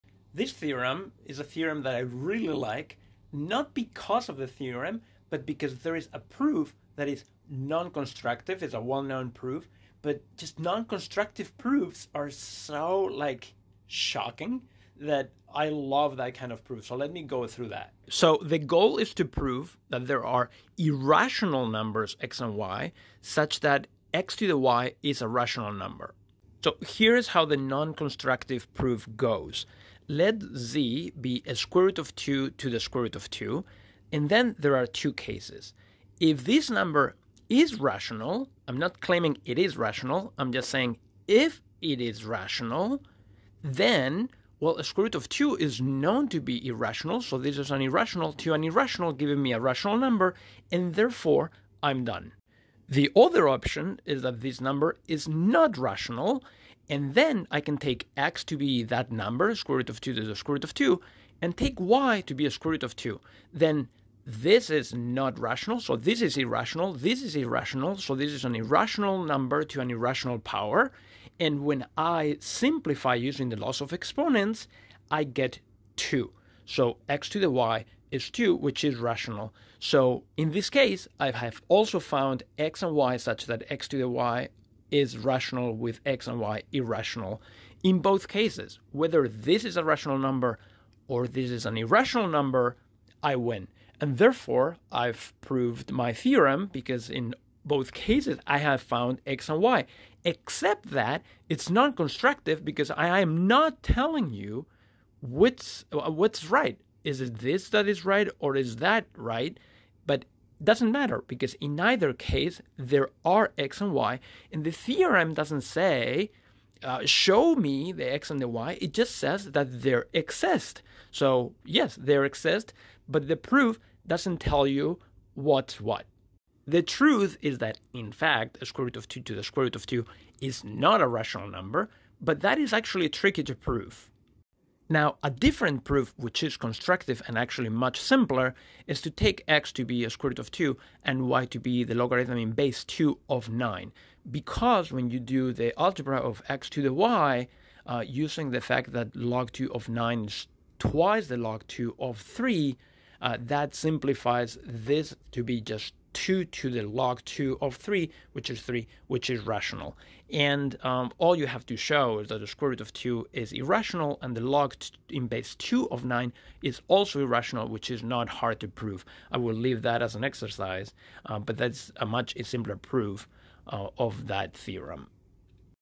Example of proof: non-constructive
In this video I show an example of a non-constructive proof (and a constructive variant).